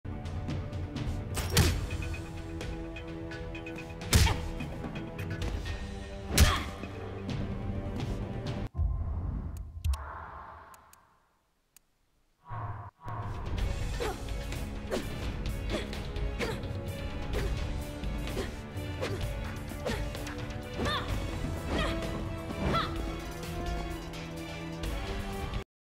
High moves example
Demonstrates how high-hitting moves can be ducked